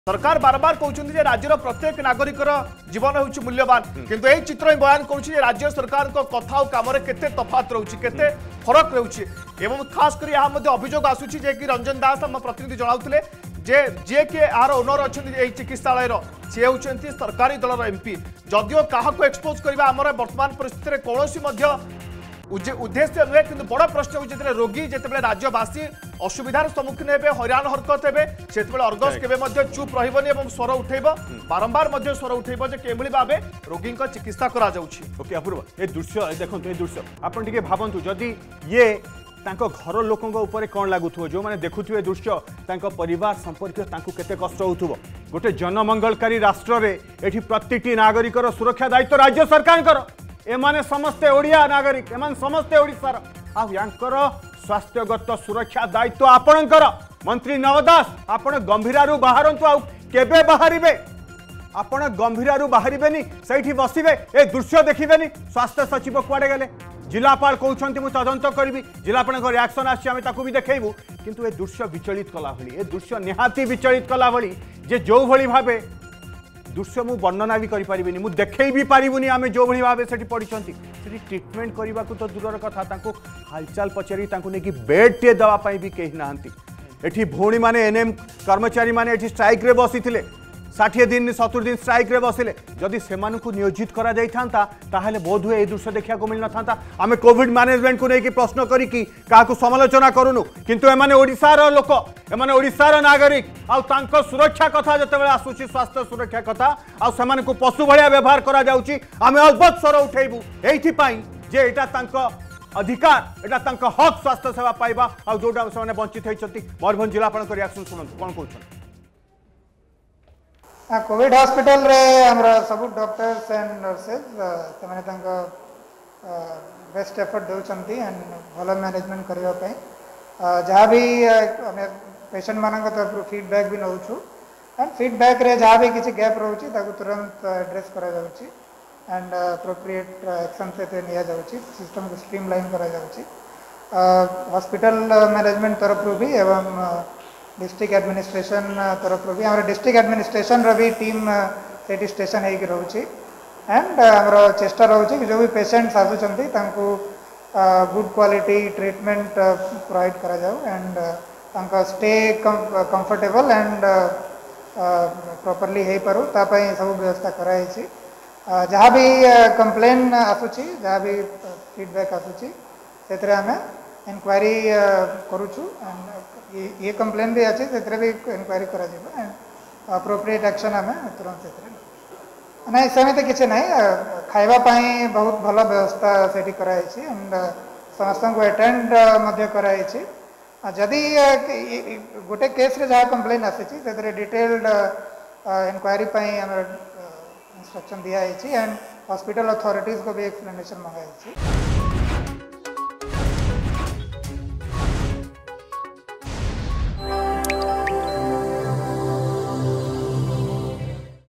ଲୋକଙ୍କ ସ୍ବାର୍ଥରେ ଅର୍ଗସ ସର୍ବଦା ସ୍ବର ଉଠାଇବ।
କୋଭିଡ ହସ୍ପିଟାଲରେ ଅତି ଶୋଚନୀୟ ଭାବରେ ପଡି ରହିଛନ୍ତି ରୋଗୀ। ଦେଖନ୍ତୁ ପୂରା ଭିଡିଓ।